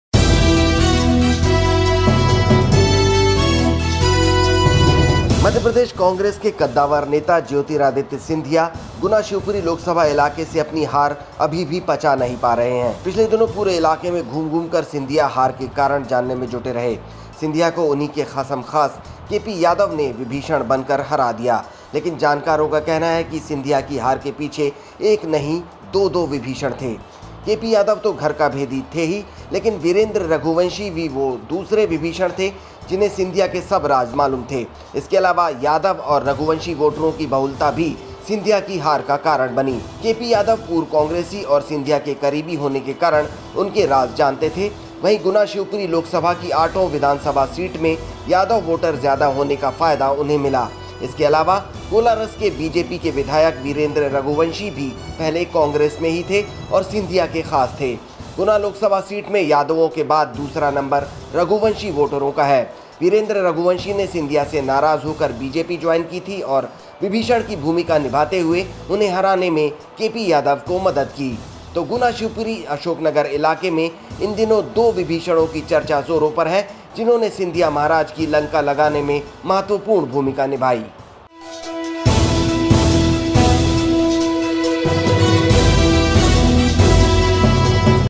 इन दो विभीषणों ने लगाई सिंधिया महाराज की लंका?
मध्यप्रदेश कांग्रेस के कद्दावर नेता ज्योतिरादित्य सिंधिया गुना शिवपुरी लोकसभा इलाके से अपनी हार अभी भी पचा नहीं पा रहे हैं। पिछले दिनों पूरे इलाके में घूम-घूम कर सिंधिया हार के कारण जानने में जुटे रहे। सिंधिया को उन्हीं के खासमखास केपी यादव ने विभीषण बनकर हरा दिया। लेकिन जानकारों का कहना है कि सिंधिया की हार के पीछे एक नहीं दो-दो विभीषण थे। केपी यादव तो घर का भेदी थे ही लेकिन वीरेंद्र रघुवंशी भी वो दूसरे विभीषण थे जिन्हें सिंधिया के सब राज मालूम थे। इसके अलावा यादव और रघुवंशी वोटरों की बहुलता भी सिंधिया की हार का कारण बनी। केपी यादव पूर्व कांग्रेसी और सिंधिया के करीबी होने के कारण उनके राज जानते थे वहीं गुना शिवपुरी लोकसभा की आठों विधानसभा सीट में यादव वोटर ज्यादा होने का फायदा उन्हें मिला। इसके अलावा कोलारस के बीजेपी के विधायक वीरेंद्र रघुवंशी भी पहले कांग्रेस में ही थे और सिंधिया के खास थे। गुना लोकसभा सीट में यादवों के बाद दूसरा नंबर रघुवंशी वोटरों का है। वीरेंद्र रघुवंशी ने सिंधिया से नाराज होकर बीजेपी ज्वाइन की थी और विभीषण की भूमिका निभाते हुए उन्हें हराने में केपी यादव को मदद की। तो गुना-शिवपुरी-अशोकनगर इलाके में इन दिनों दो विभीषणों की चर्चा जोरों पर है जिन्होंने सिंधिया महाराज की लंका लगाने में महत्वपूर्ण भूमिका निभाई।